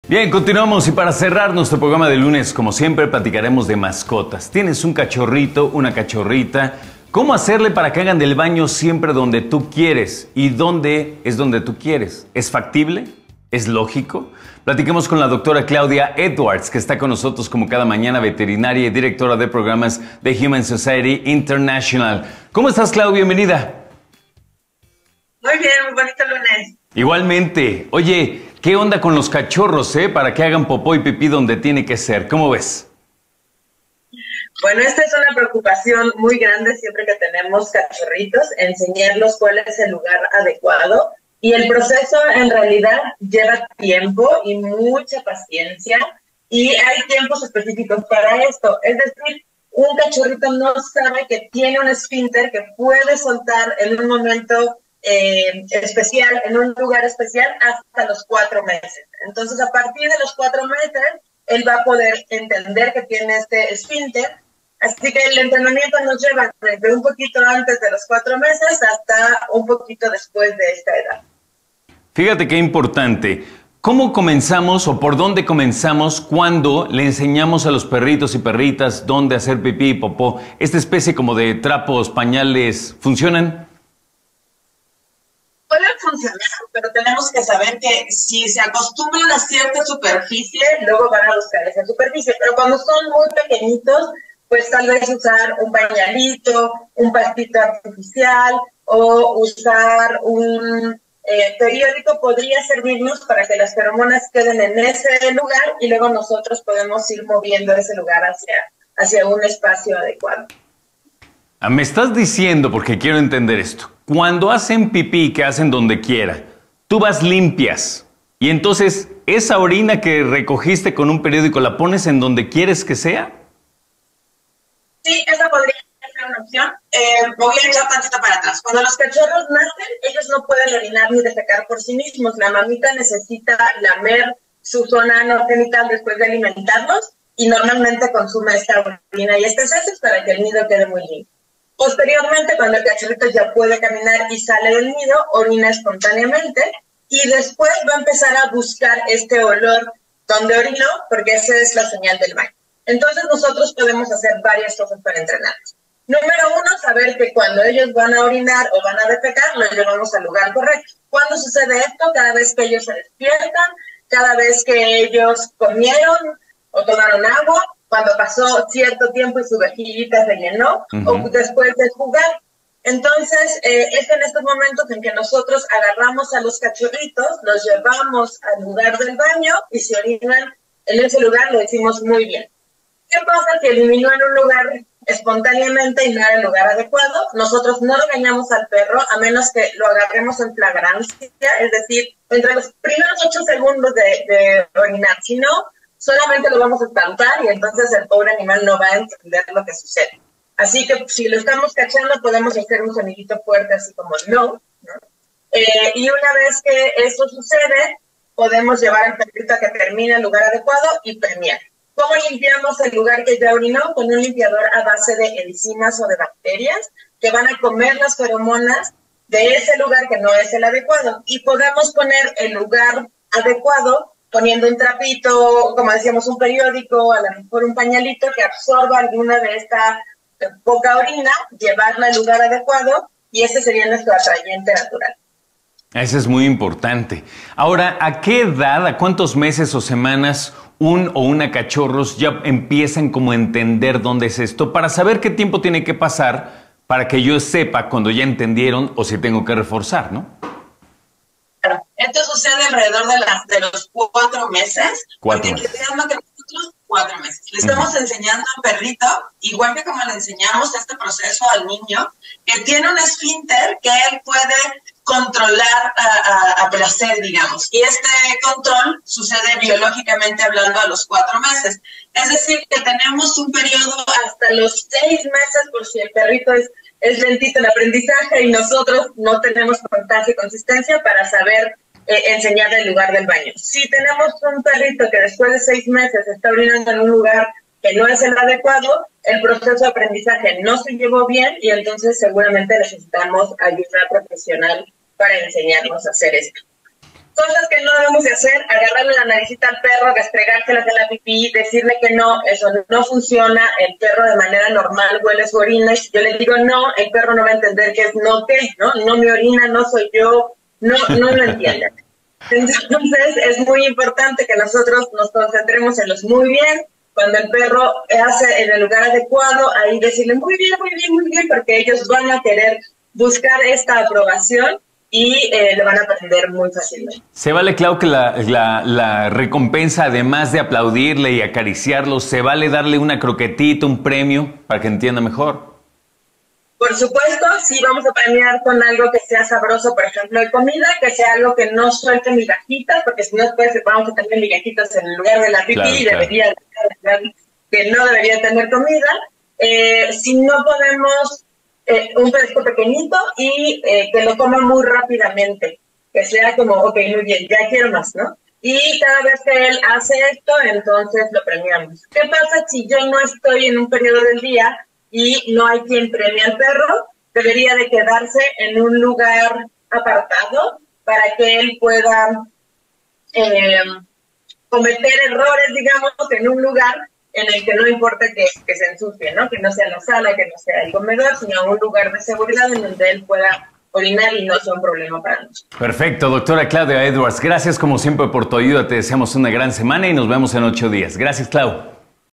Entrenamiento de baño para cachorros
Aprende cómo enseñar a tu cachorro a hacer del baño en el lugar correcto. La veterinaria Claudia Edwards te comparte los mejores consejos.

Sintonízanos aquí ViBETV
⏰: 10:00 am